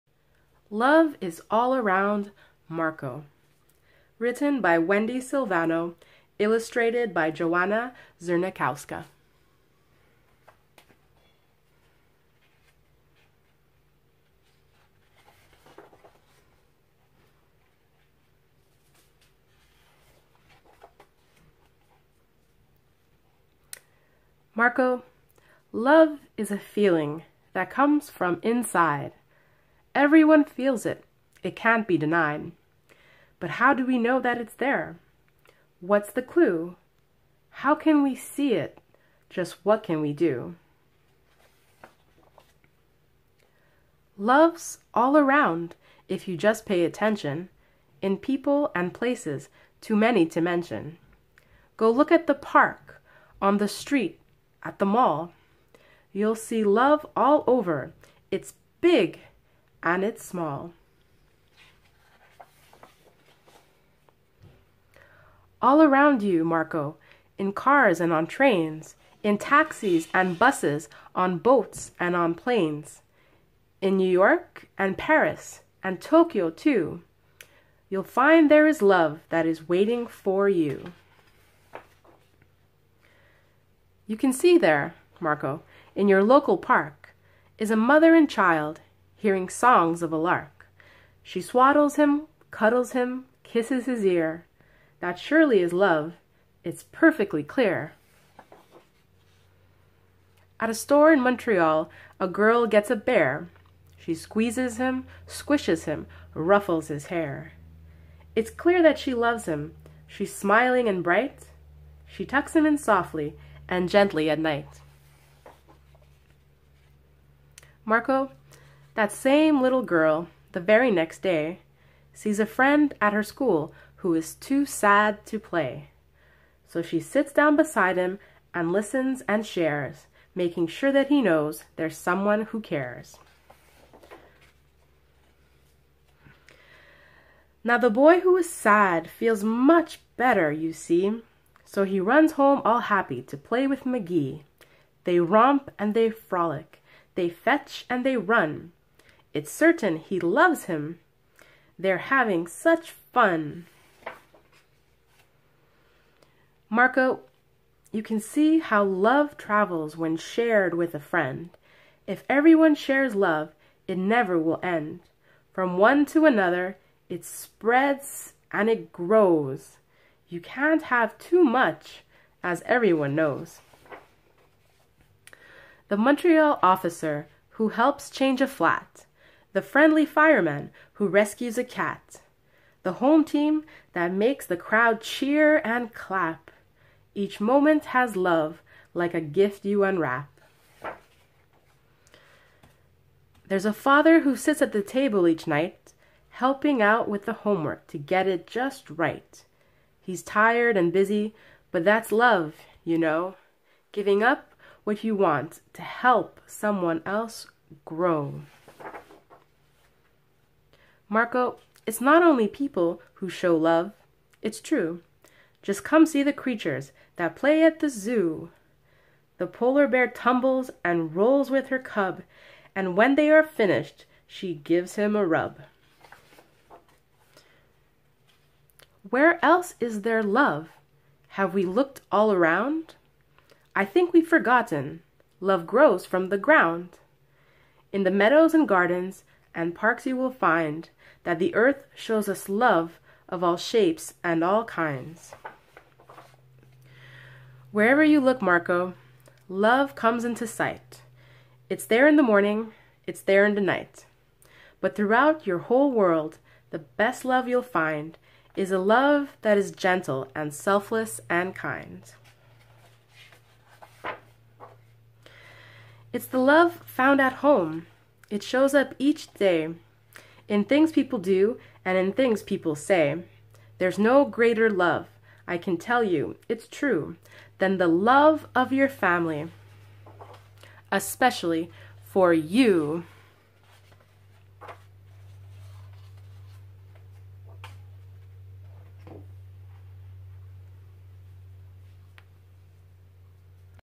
LOVE IS ALL AROUND THE WORLD read aloud
"Love is All Around The World" by Wendi Silvano. Read aloud. (For Marco)
* All material is owned by the creator mentioned in this video and the description. *

I regularly upload videos of books read aloud for kids.